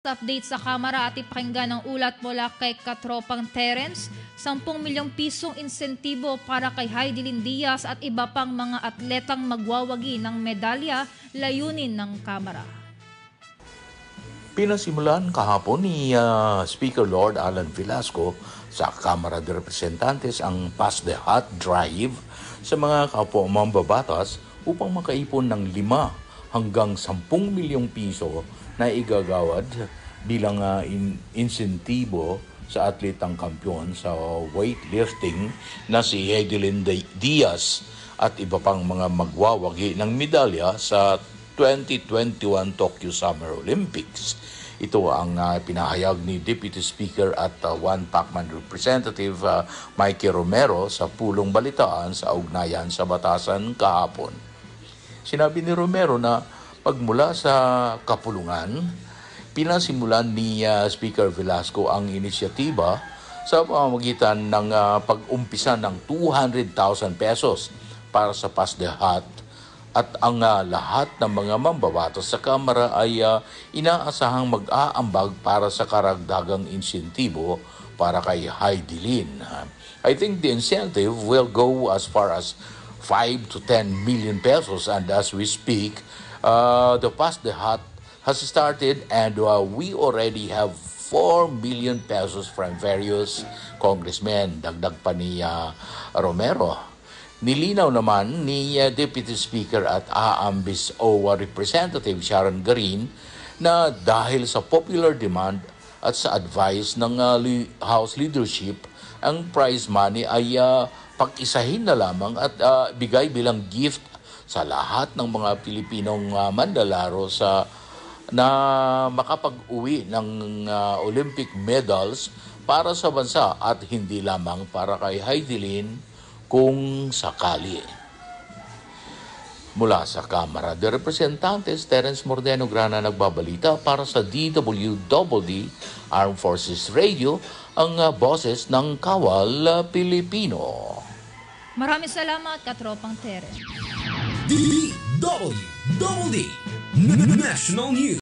202107291215 report at AFP Radio
P10M INSENTIBO PARA KAY HIDILYN DIAZ AT IBA PANG MGA ATLETA NA MAGWAWAGI NG MEDALYA, LAYUN NG KAMARA